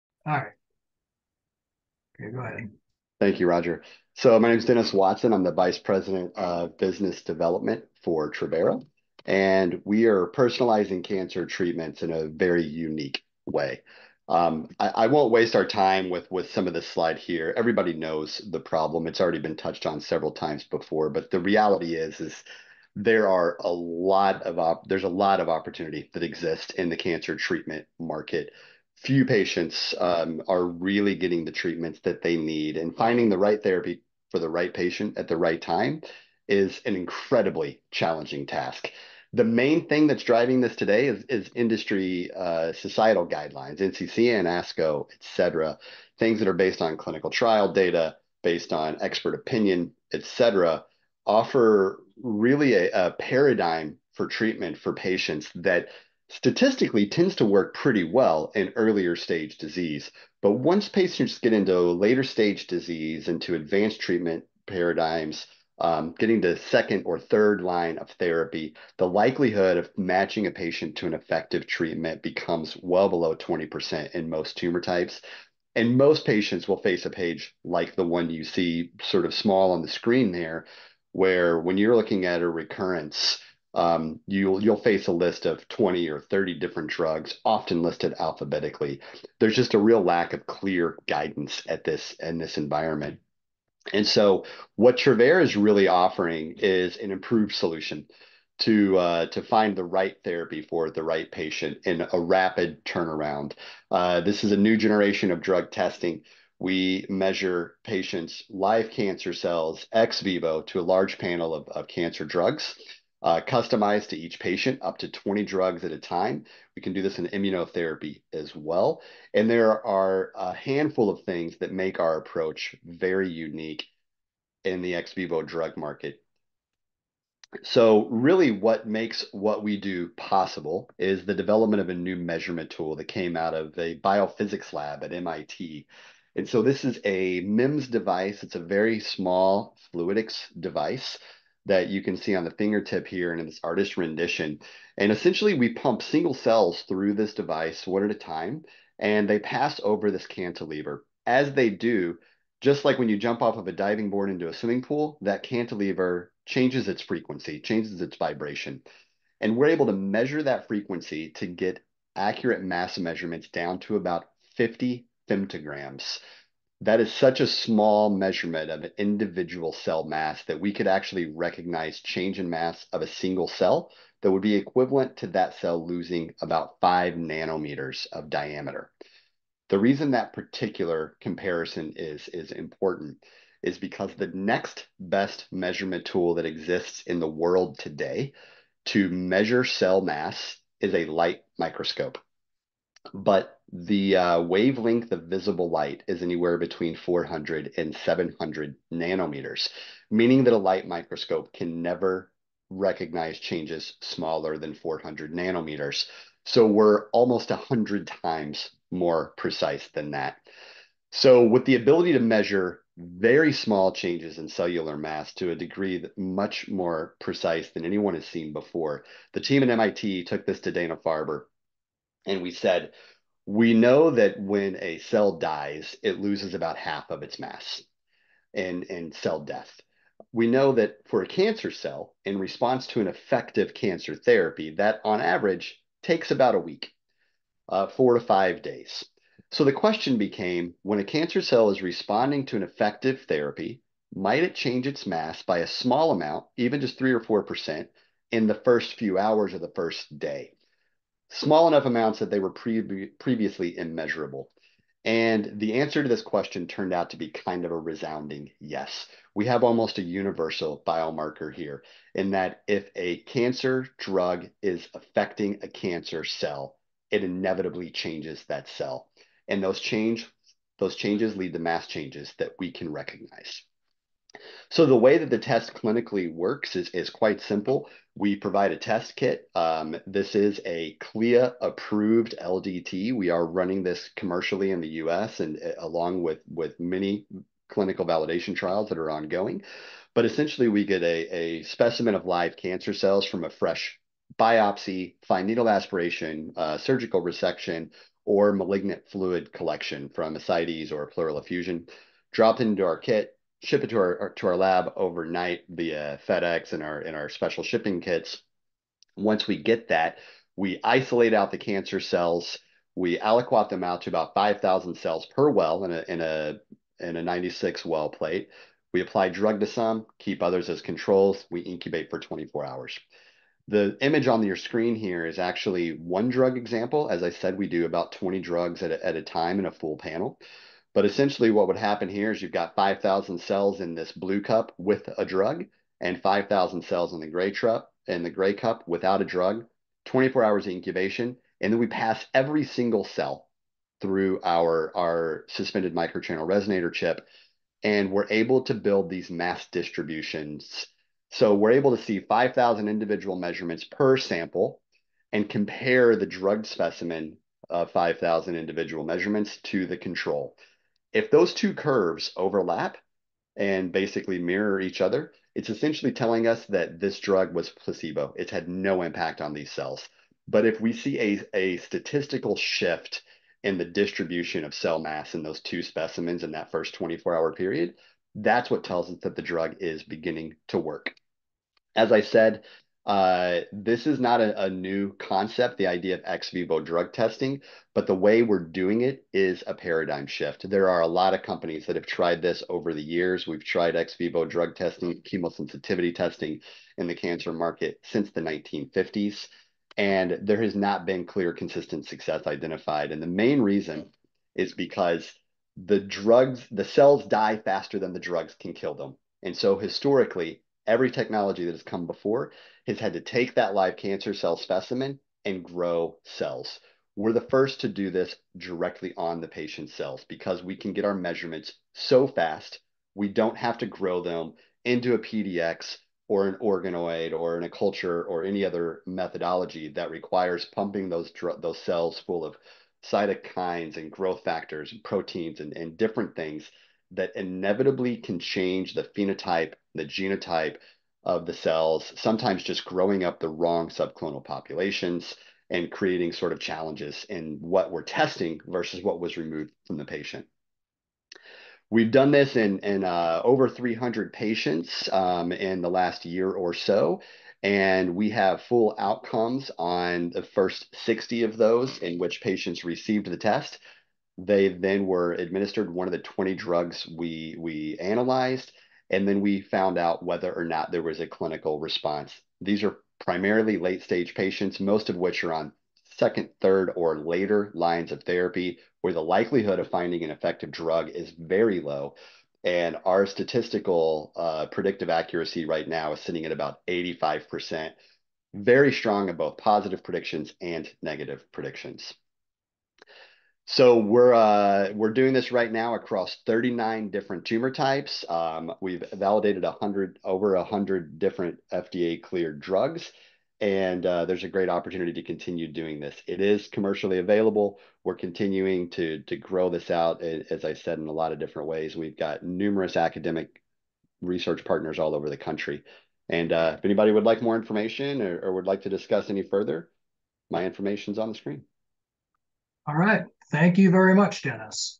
Dennis Watson, Travera
Travera can deliver a test report to the ordering physician two days after sample collection. Their lab tests a panel of FDA-approved drugs on live cancer cells. In early studies, the Travera test matched the actual responses of 80% of treatments.